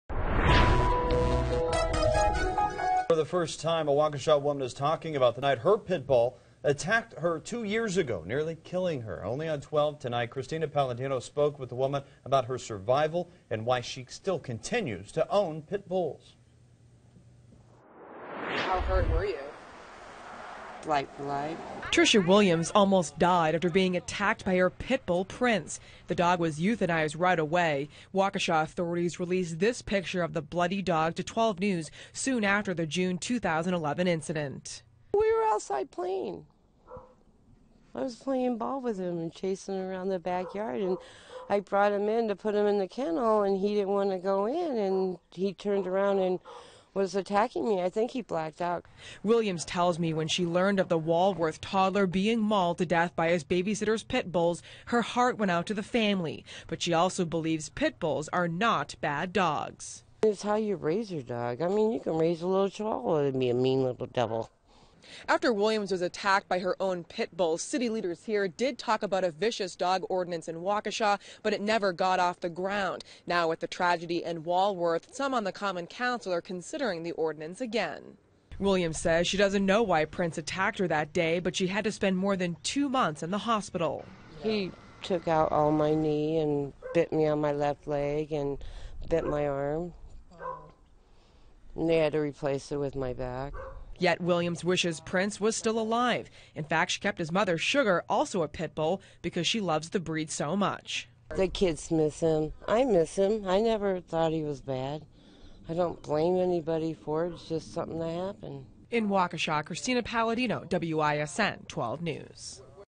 Only on 12: Waukesha woman talks about pit bull attack in 2011
A Waukesha woman is talking about the night her pit bull attacked two years ago.